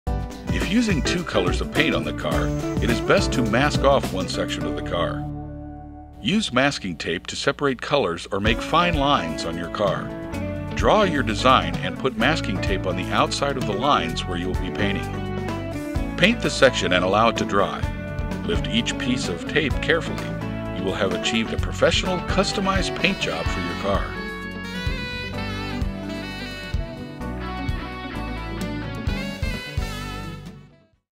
Painting Idea | Pinewood Racing Car | PineCar Derby
Quick and easy idea that lets your paint your pinewood racing car with precision and ease.